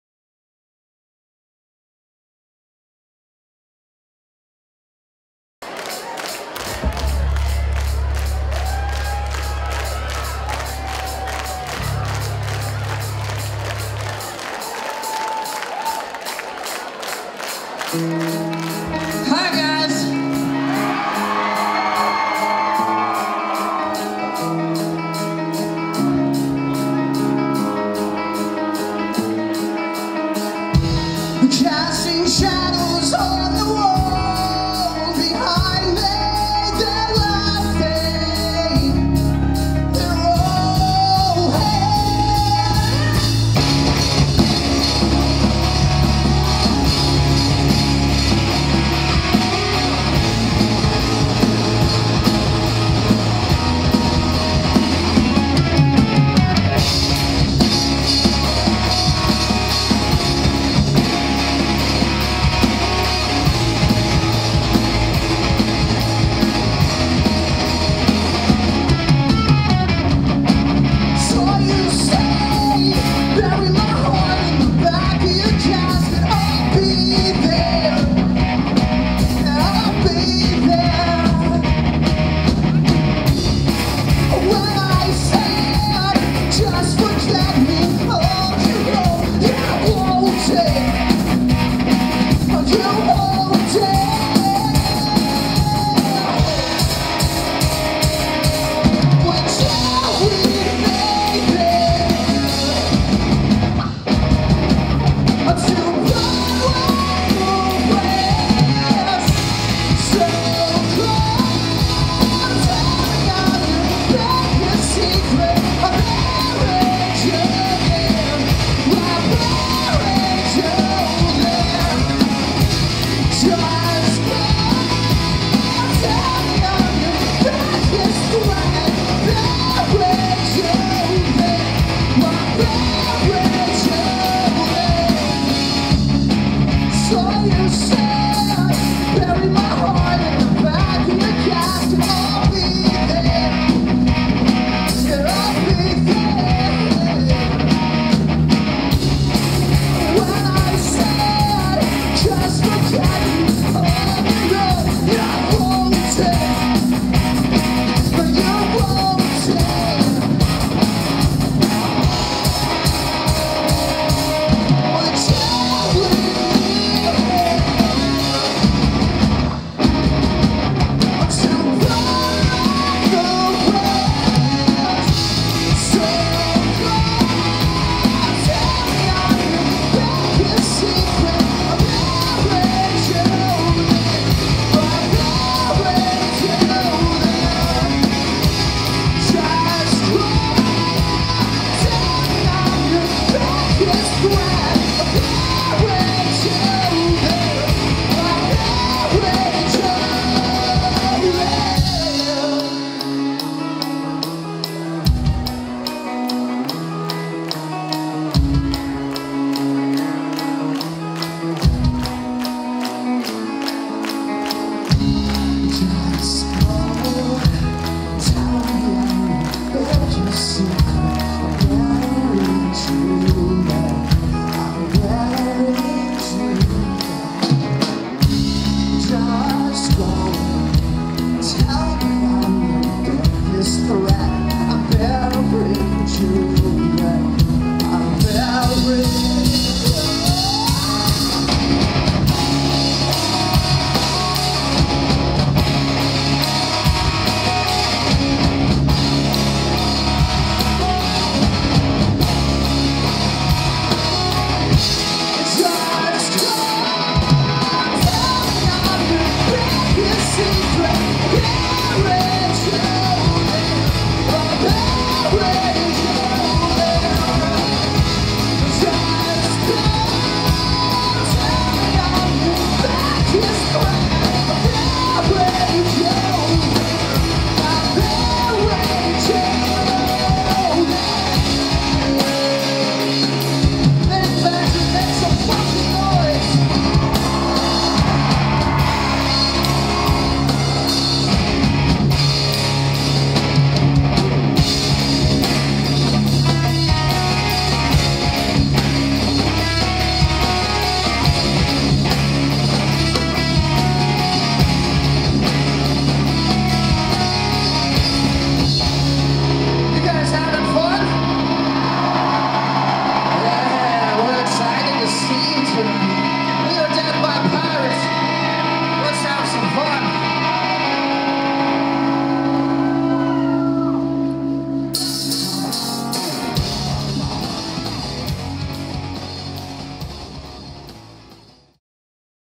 Death by Pirates Plays The Knitting Factory
Death by Pirates plays at the Knitting Factory on Inauguration Day 2017.